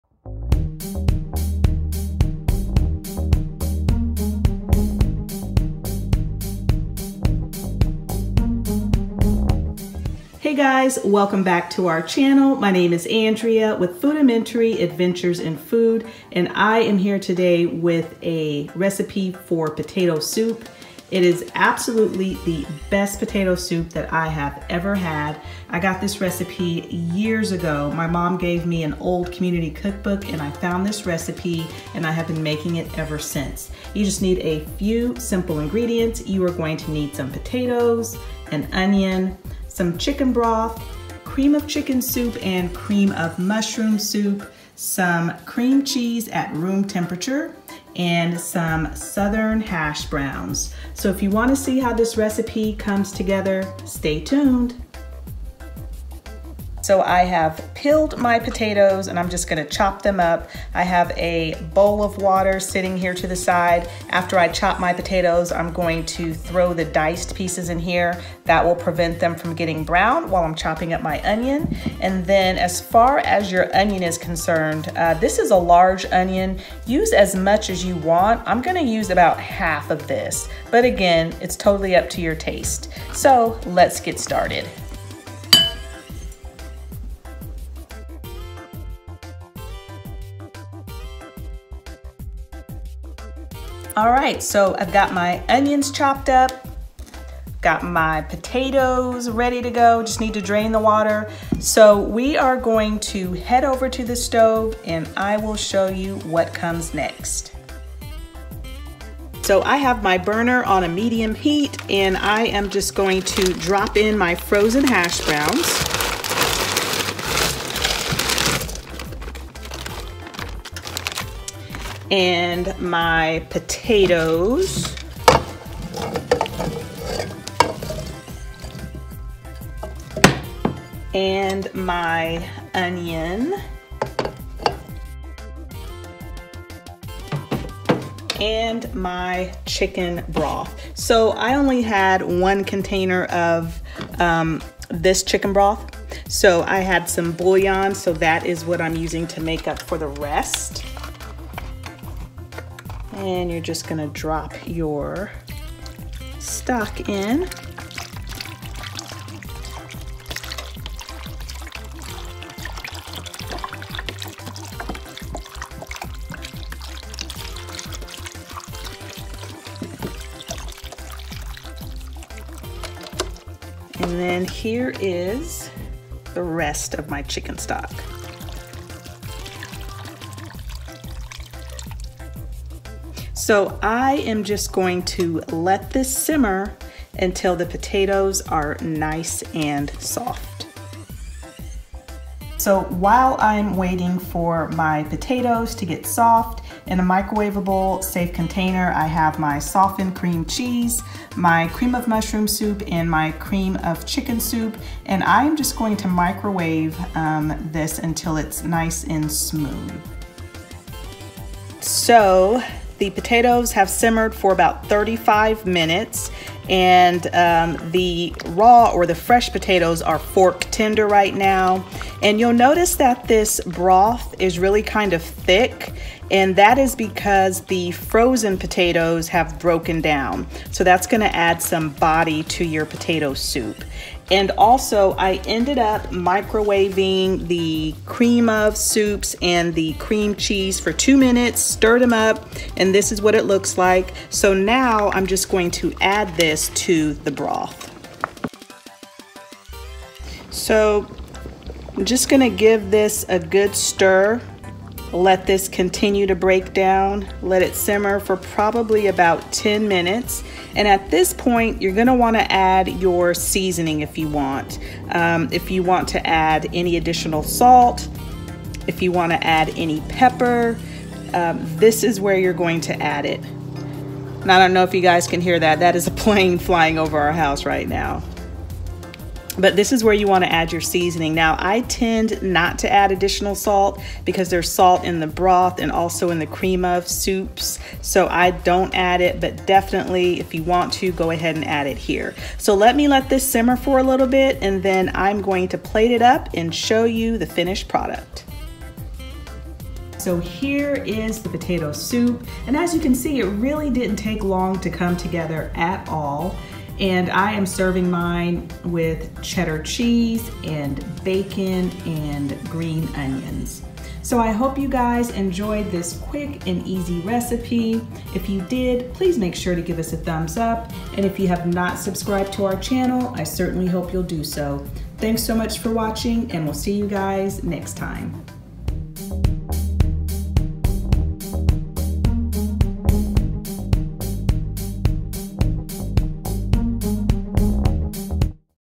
Potato Soup Recipe | Quick and Easy Weeknight Recipe | Delicious and Hearty Soup!
Potato Soup
6 Cups of chicken broth
1- 32 oz package of Southern hash browns
3 medium potatoes, diced
1 onion to taste
1- 8 oz block of cream cheese, softened
1 can of cream of chicken soup
1 can of cream of mushroom soup

Cook potatoes, hash browns and onions in chicken broth until tender (about 30 mins). In microwavable bowl, mix soups and cream cheese together until softened (about 2 minutes). When potatoes are tender, add soup mixture and cook until well combined and warmed through. Top soup with bacon, cheese, sour cream, and green onions if desired.

Note: Soup will thicken when refrigerated.  Add milk or broth to thin it out if desired.